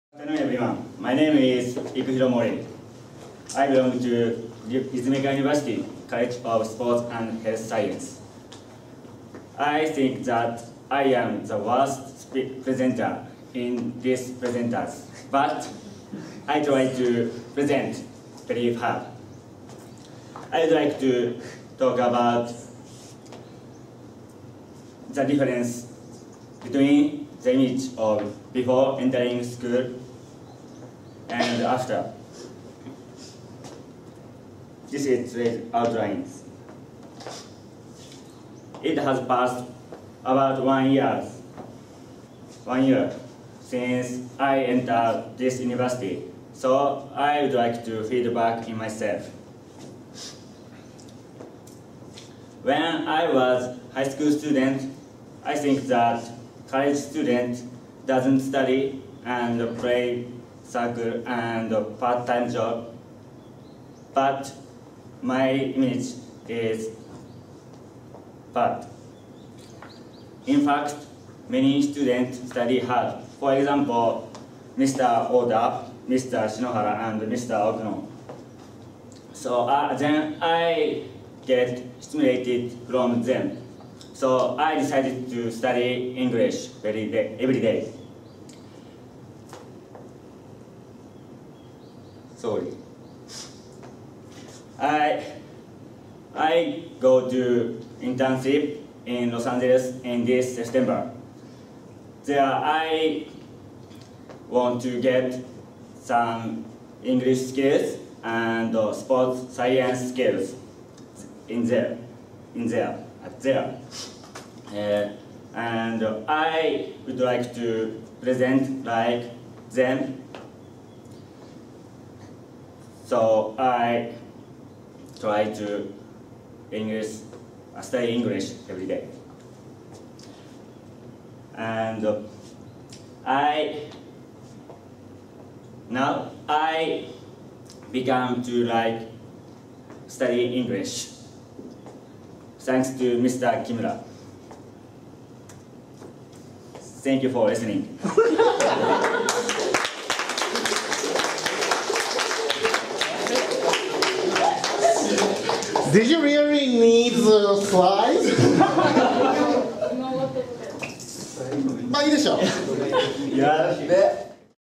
P5 Session #02: 4th Presentation by Ikuhiro MORII
「P5」は、立命館大学スポーツ健康科学部の有志学生らによる英語プレゼンテーション・ワークショップです。詳しくは、 をご覧ください。